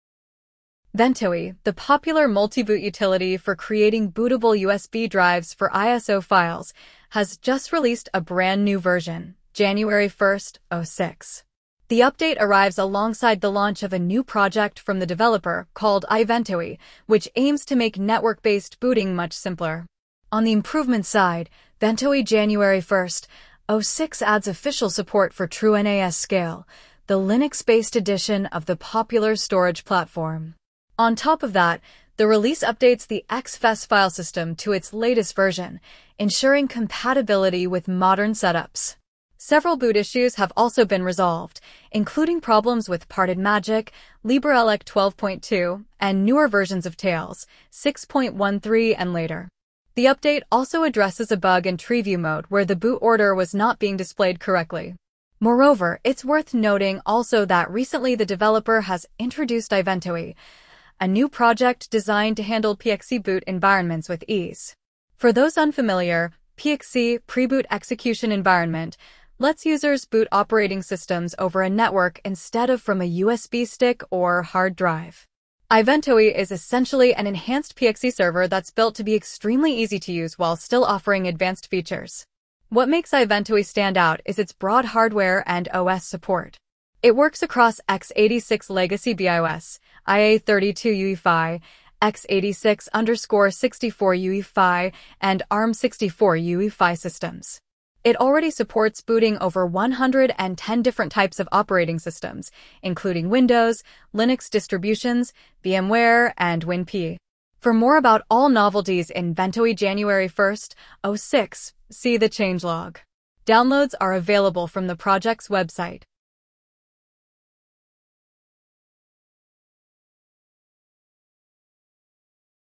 Ventoy 1.1.06 Brings TrueNAS Scale Support
Ventoy 1.1.06, an open-source tool for creating a bootable USB drive to load multiple ISO files, adds TrueNAS Scale support, updates XFS, and fixes boot issues.